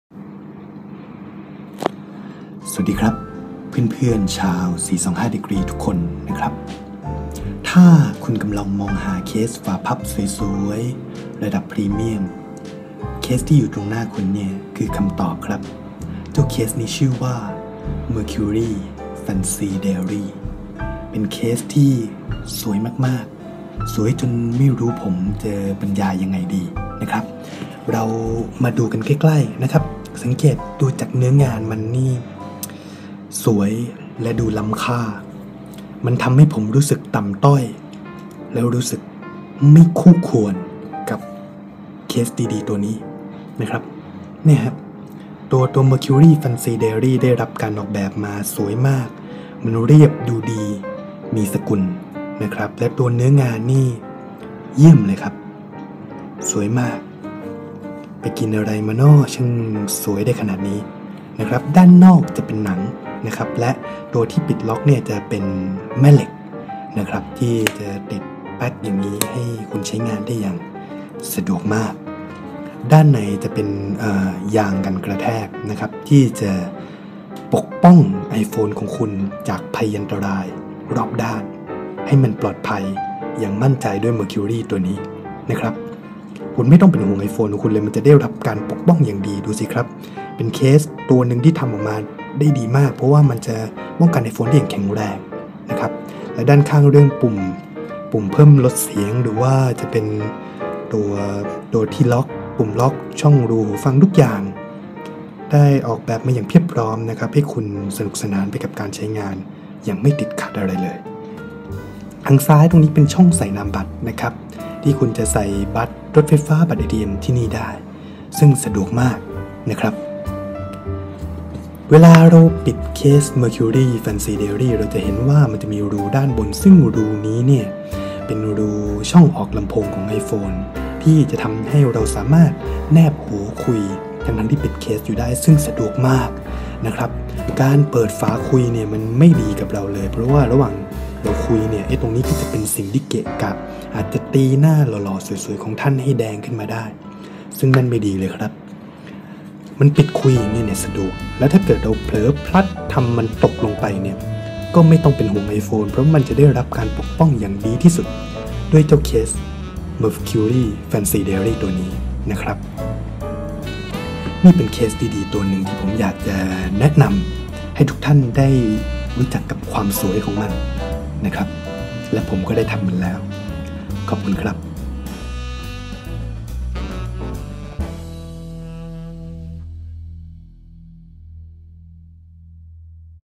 Review เคส Mercury Fancy Diary (เสียงหล่อโดย 425degree)
รีวิวเคส Mercury Fancy Diary ที่สวยเกินบรรยาย
เคสฝาพับหนังชั้นดี ในราคาประหยัด

ขอออภัยที่ภาพสั่นนะครับ มือมันตื่นเต้นที่ได้เห็นเคส Mercury